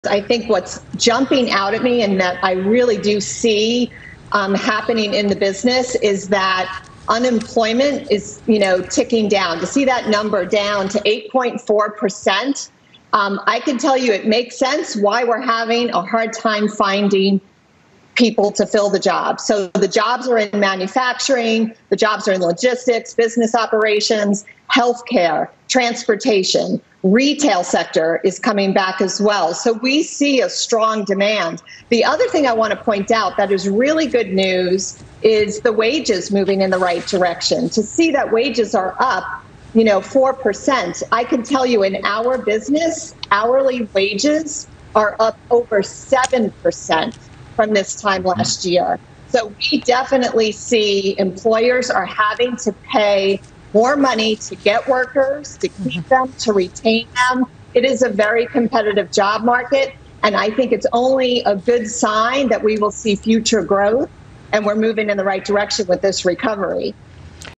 Workforce Analyst: Report “A Good Sign…We Are Moving In The Right Direction W/ This Recovery”
Fox Business Network contributor and workforce analyst Joanie Courtney calls the August 2020 jobs report “a good sign… we are moving in the right direction with this recovery.” From Fox Business coverage on 9/4/2020.